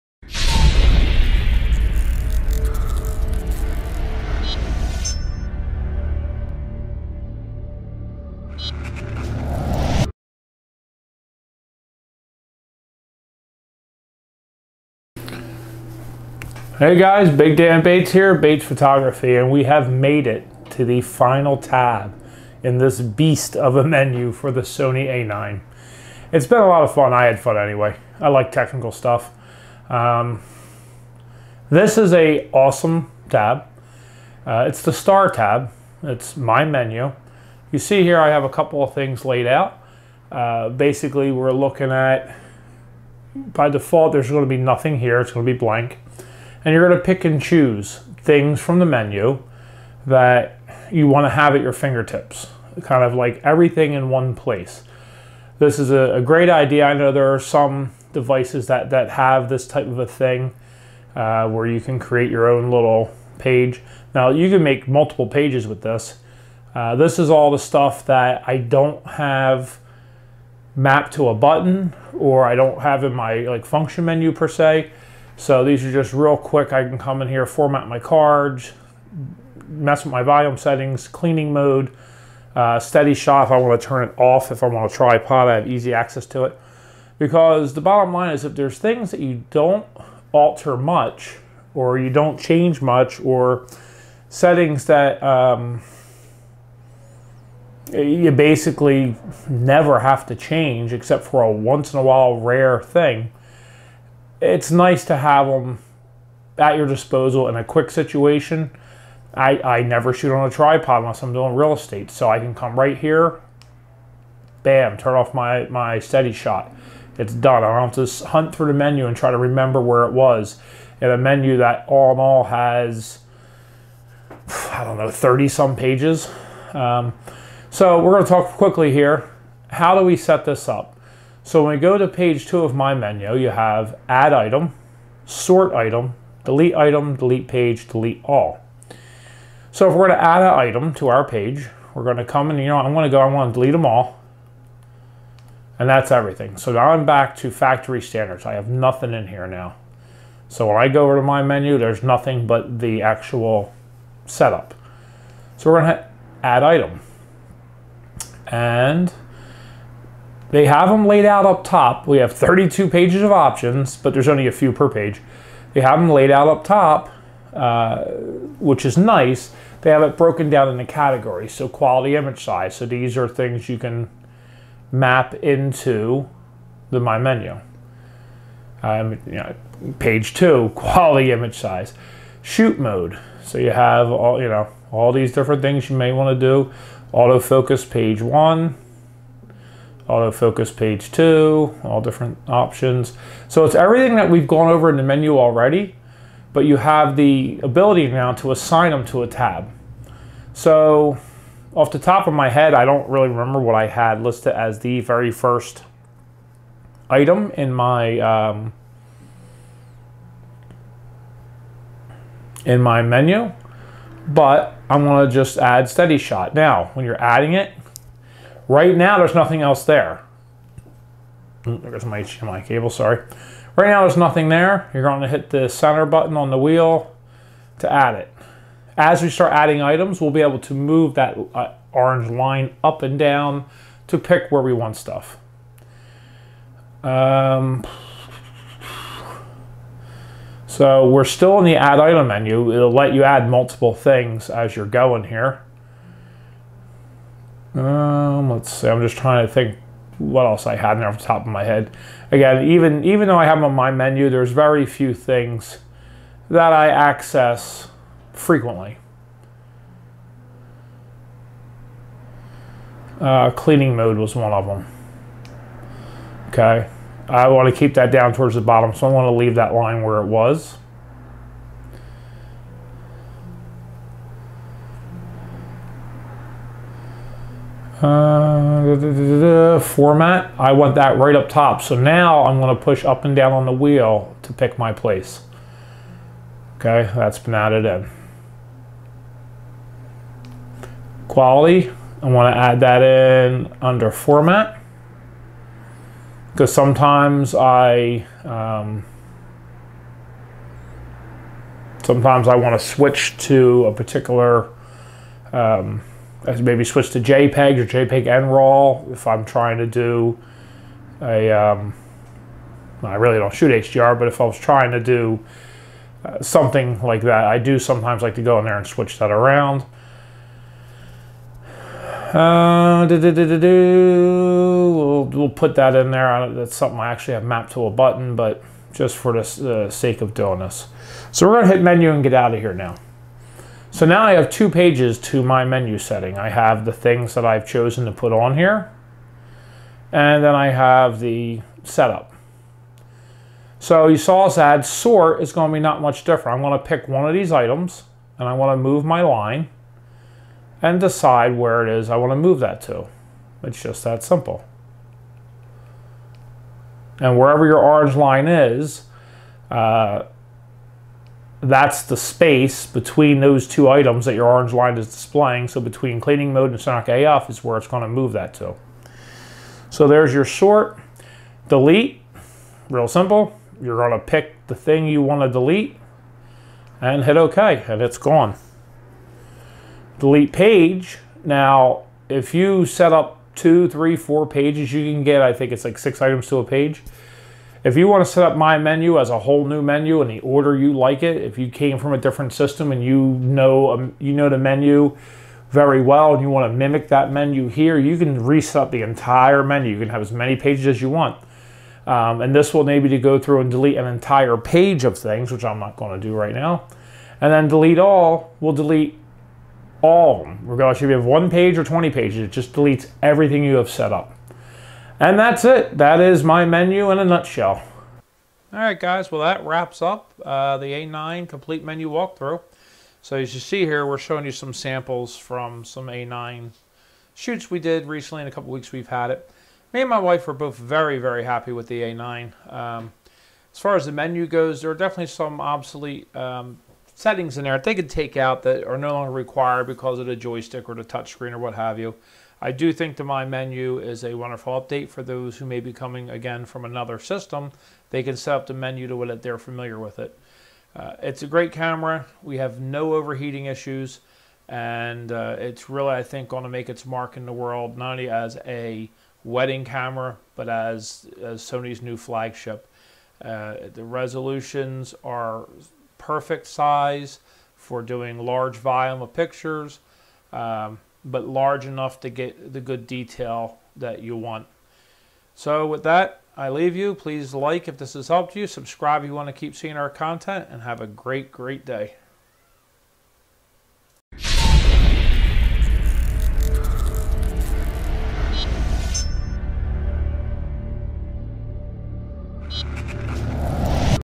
Sony a9 Pt 7 : My Menu Tab : Final Thoughts : Sample Images
Finishing the Sony a9 Complete Menu Walkthrough strong with one of the coolest new menu items, MY Menu. Setup your own customized menu with only the items you frequently access all on one tab or recreate the entire menu in an order that more closely looks like what you know from Cannon, Nikon, Fuji, Olympus, or Pentax. Then enjoy some sample images while I give my final thoughts.